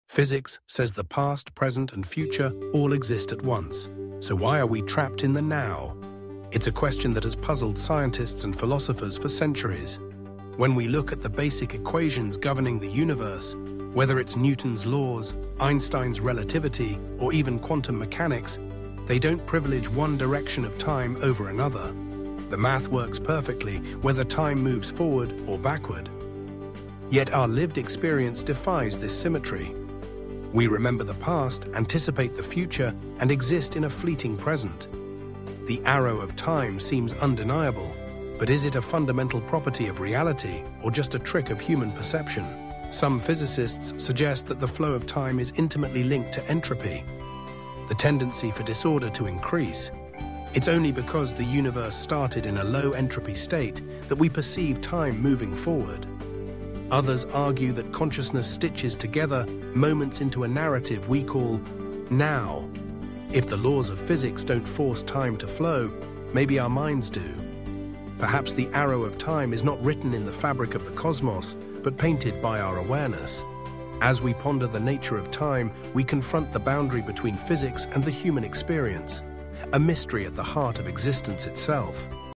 #80. The Black Hole 'No-Hair' Theorem, Do All Black Holes Look the Same
A theorem states that a black hole is defined only by its mass, charge, and spin-it has no other 'hair.' But is this true? Finding 'hair' would challenge our understanding of gravity and information. 6